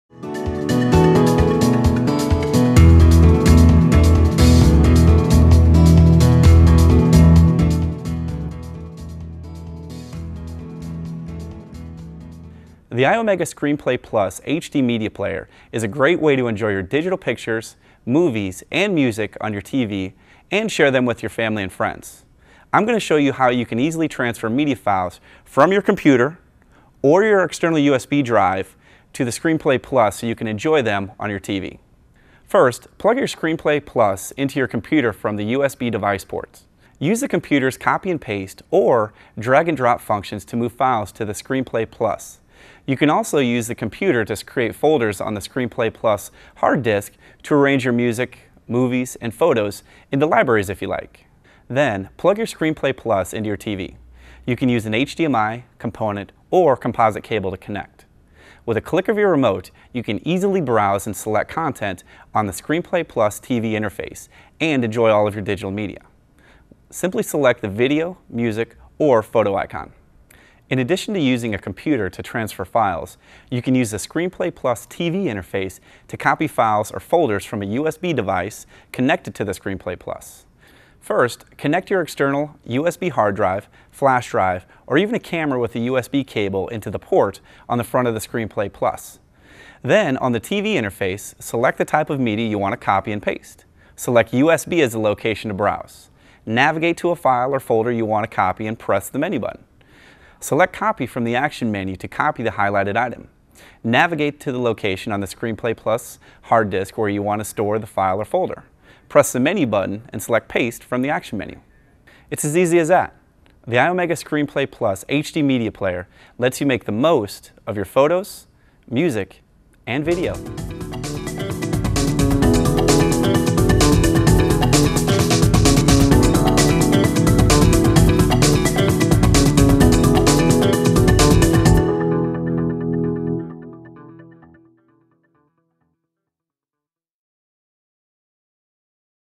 Transfer Media Files to Your Iomega ScreenPlay Plus
You can easily transfer media files from your computer or external USB drive to your ScreenPlay Plus HD Media Player so you can enjoy them in high-definiton on your TV.